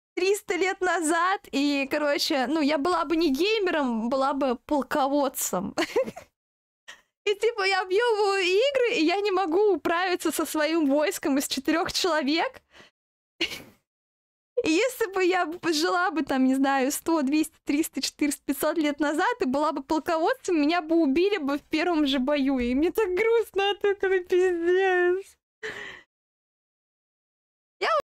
СЕГОДНЯ СТРИМИТ МОЯ ДОБРАЯ СЕСТРА-БЛИЗНЕЦ, ОНА АБСОЛЮТНО НЕТОКСИЧНАЯ / zefirooq
Клип с твича от rnnmll

[SparklyRichInternPogChamp-vQHkFFMl3k9uCye5]